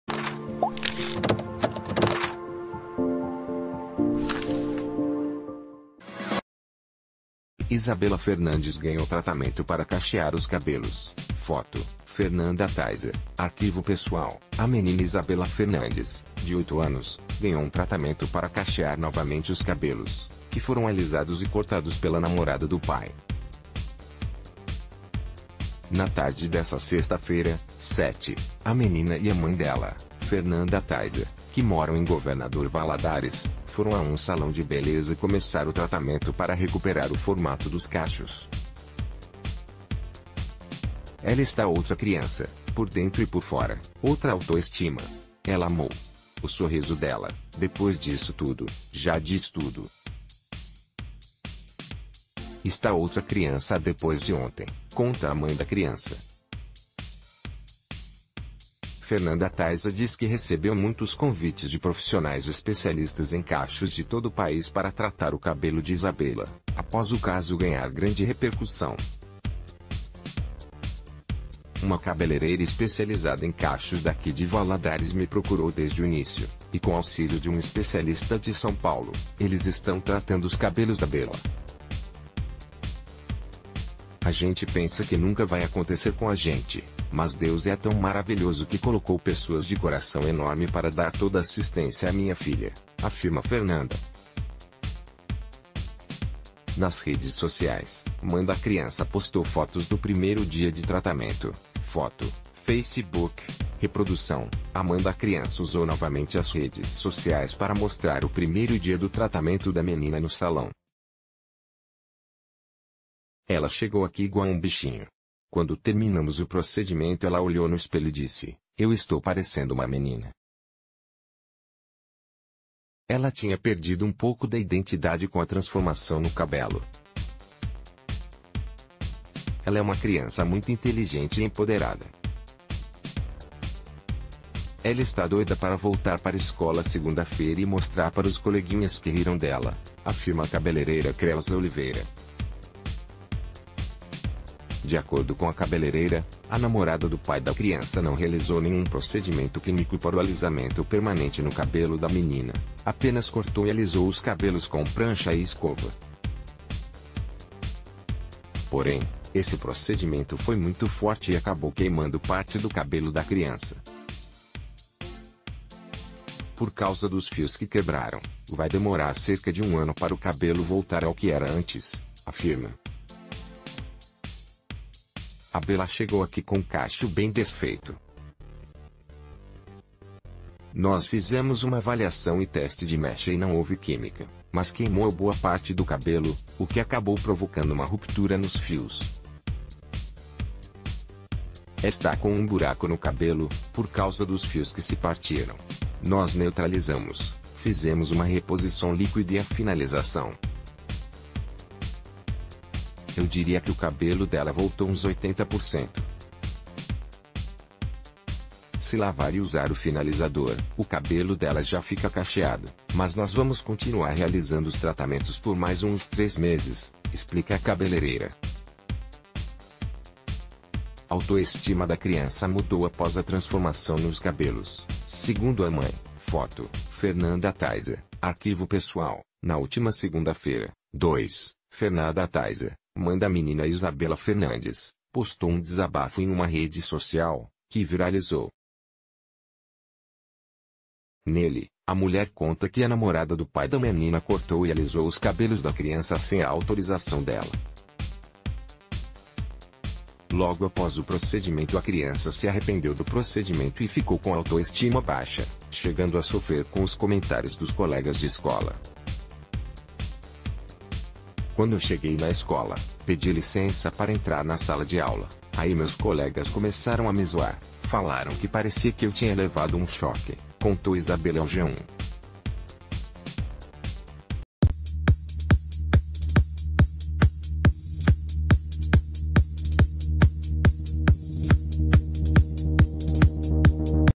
Menina de Governador Valadares que cabelo alisado pela namorada do pai ganha tratamento e volta a...
Menina de Governador Valadares que cabelo alisado pela namorada do pai ganha tratamento e volta a ter cachos